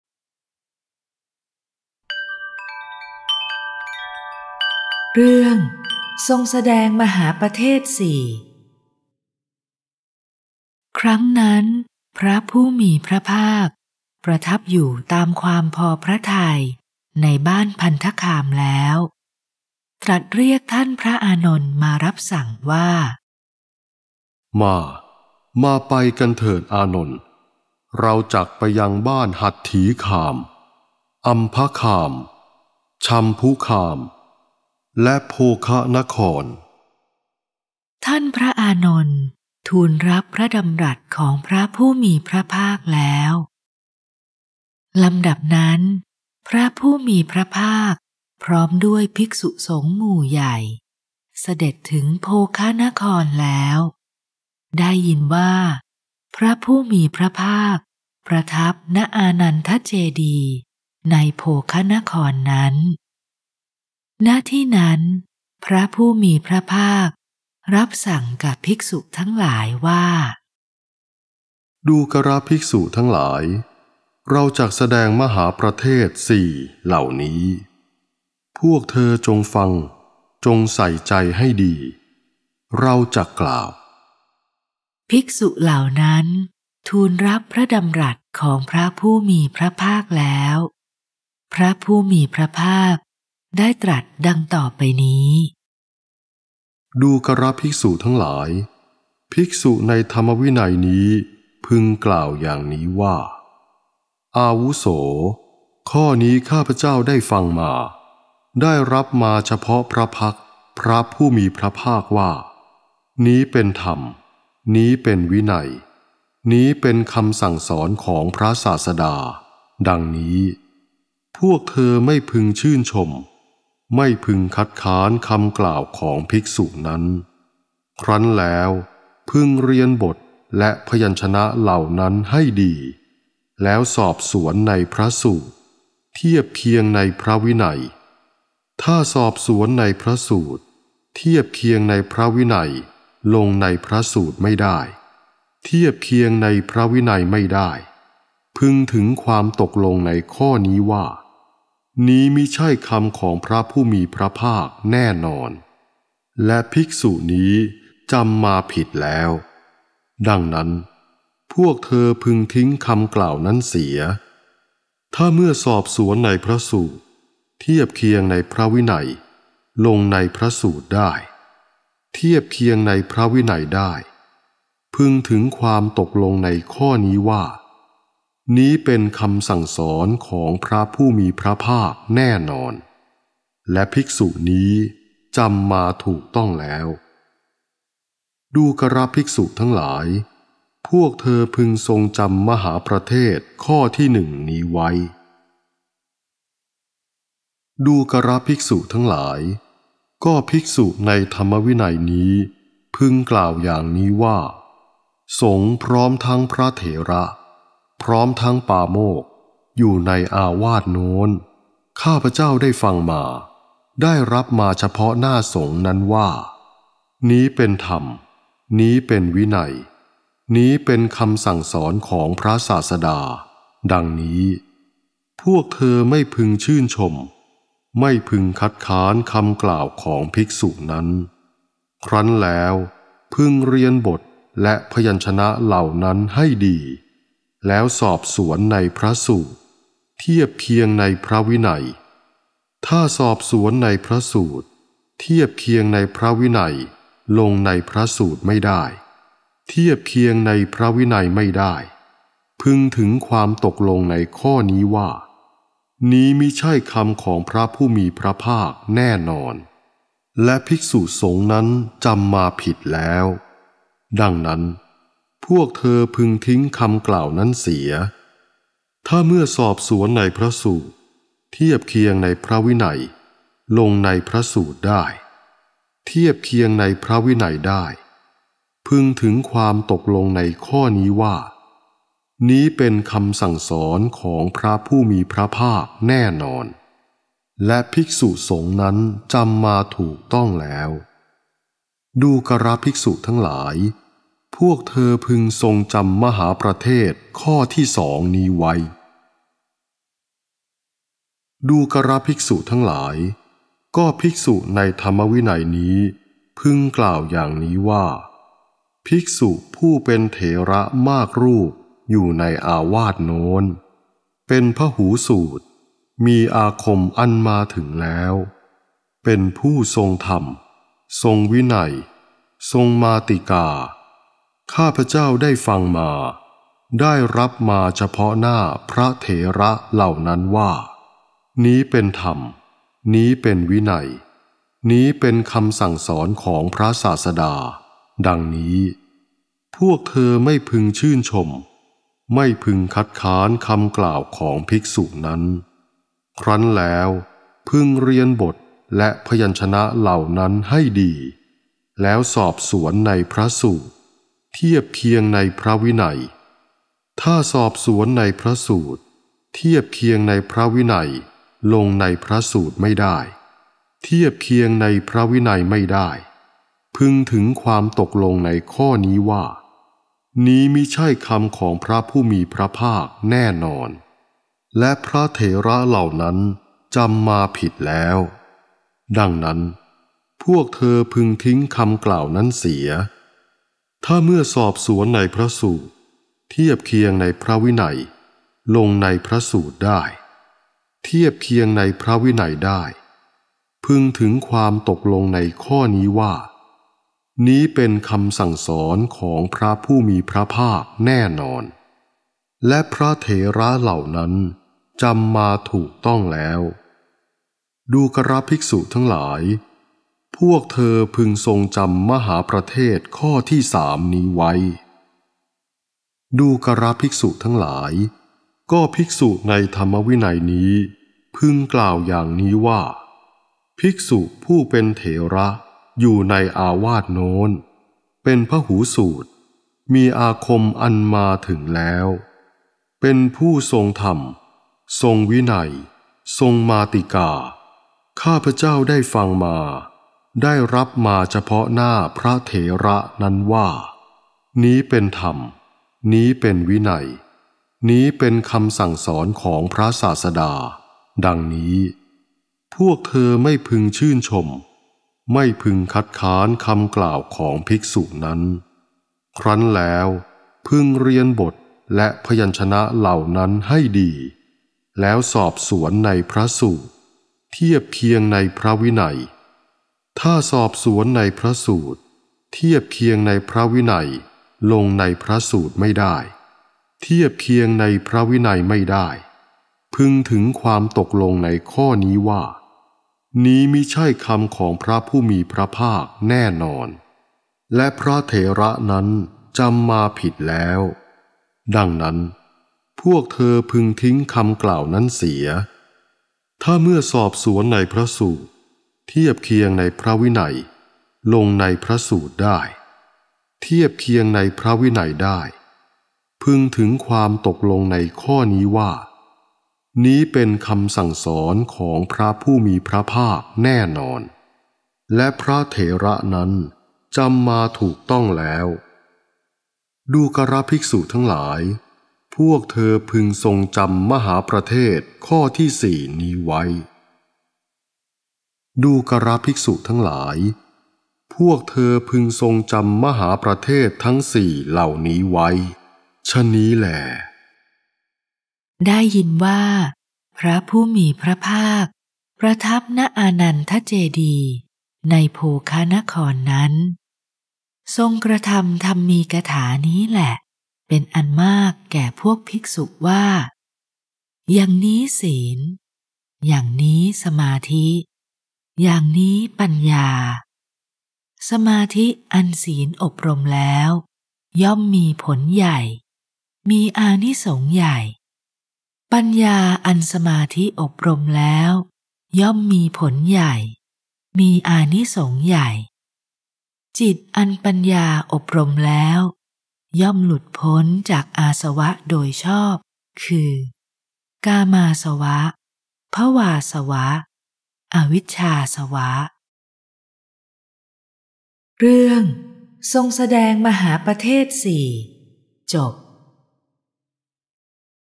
ทรงแสดงมหาปเทส ๔ อย่าง | พระพุทธกิจ พรรษาที่ ๔๑ - ๔๕
มหาปรินิพพานสูตร
พระไตรปิฎก ฉบับหลวง เล่ม ๑๐ ข้อที่ [๑๑๒] - [๑๑๖]

พระพุทธกิจ